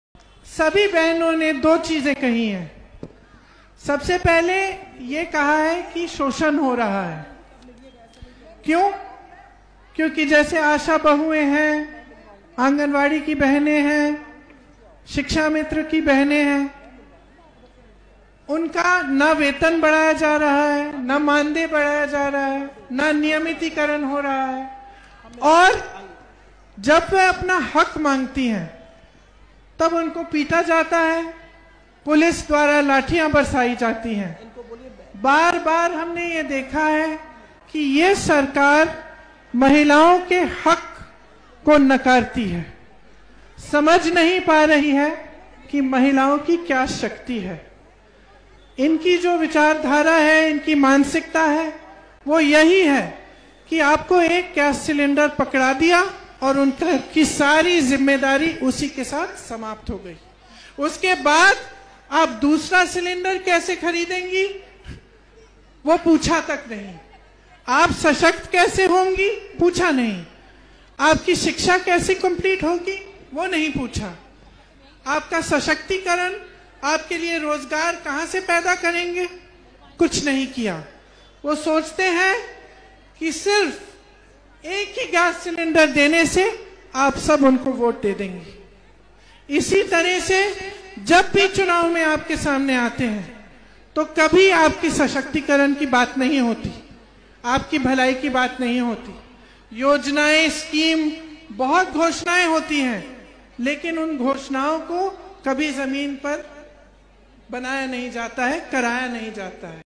महिलाओं का शोषण हो रहा है,अपना हक़ मांगने पर उन्हें मारा और पीटा जाता है।महिला_शक्ति_लायेंगी_प्रगति
महिलाओं का शोषण हो रहा है,अपना हक़ मांगने पर उन्हें मारा और पीटा जाता है।